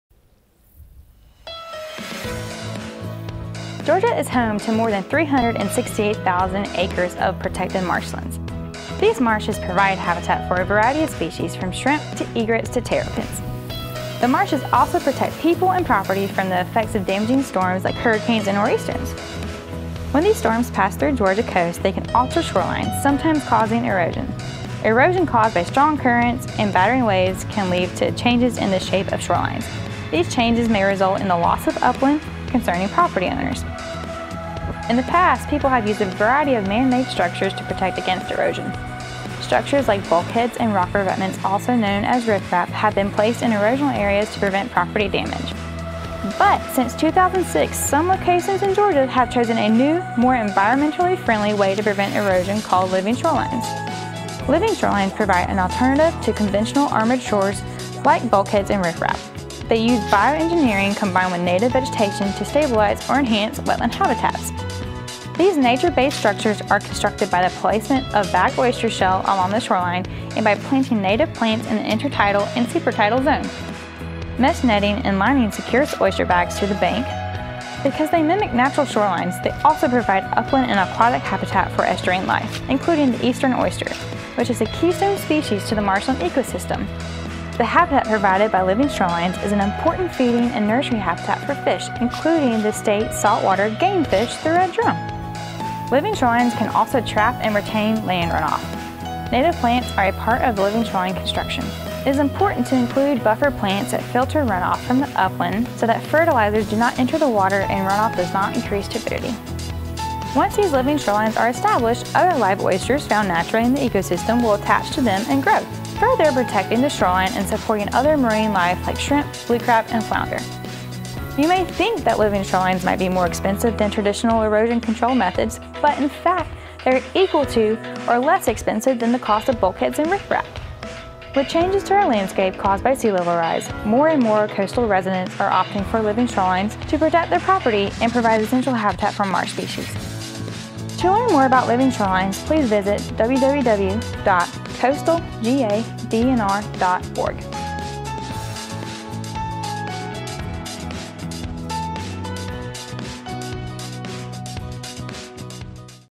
Living Shorelines presentation for Georgia Department of Natural Resources "CoastFest 2020"
The Shoreline Comes Alive: Video presentation for Georgia Department of Natural Resources - Coastal Resources Division virtual "CoastFest 2020".

The CoastFest 2020 educational program targeting middle to high school aged students was presented in a livestream format with this pre-produced insert.

For this presentation I interviewed Coastal Resources Division Wetlands Biologist Jordan Dodson to develop a script that interpreted her mission for students.  I acted as videographer for original content, directed, recorded and edited Jordan's voiceover, sourced stock footage, and transcribed the captioning.

I was also Assistant Director for the livestream webcast to the Coastal Resources Division's website, YouTube and Facebook.

GADNR-CRD retains all rights to these presentations and are presented here only as a demonstration of my production capabilities.